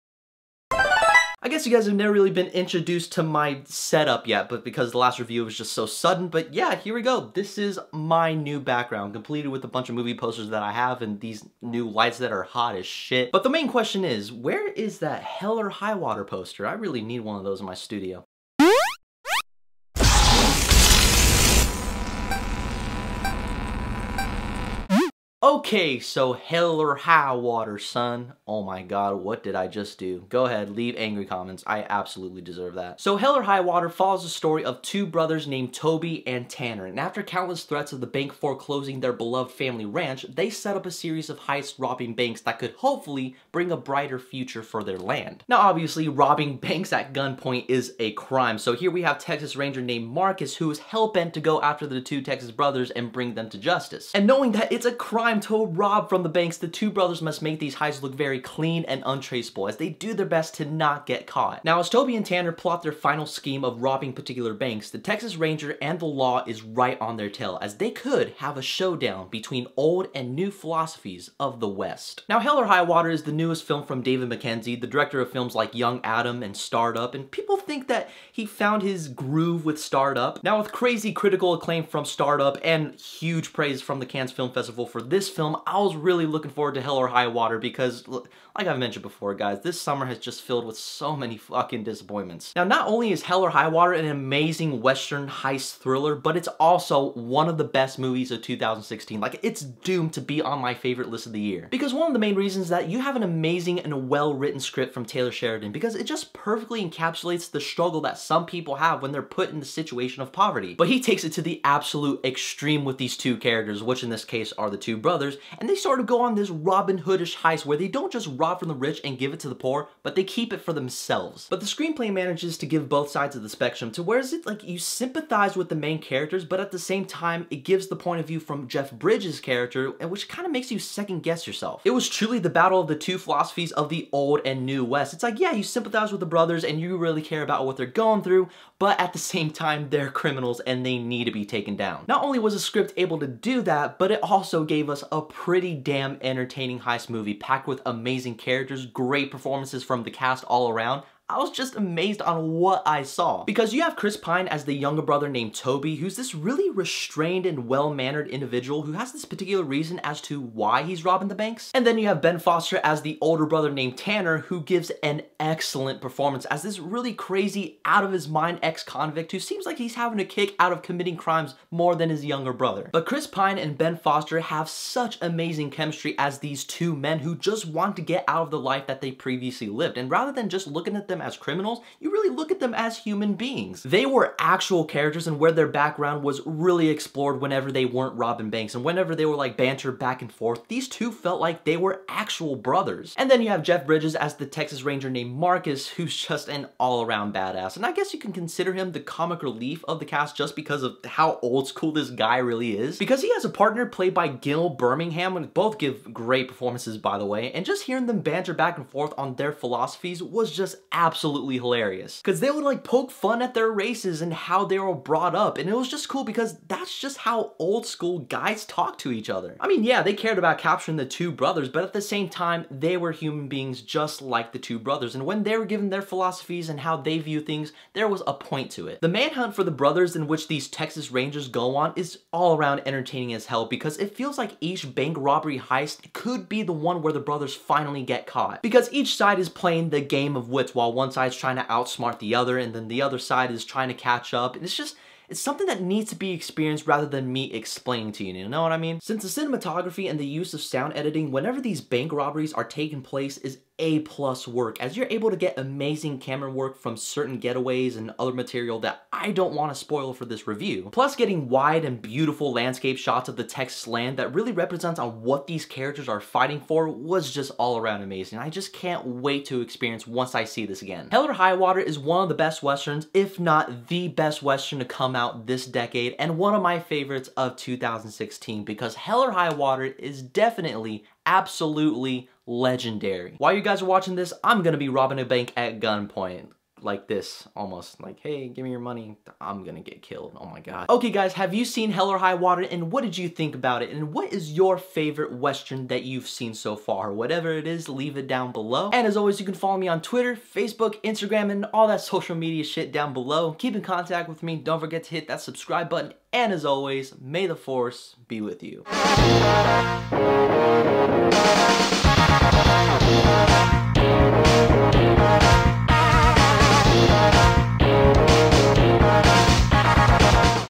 Hell or High Water - Movie Review
Committing a series off bank robberies, Texas brothers, Toby and Tanner, are now on the run from the law which could ultimately lead to the showdown of Old and New West philosophies. Here's my review of 'HELL OR HIGH WATER'! Enjoy! :)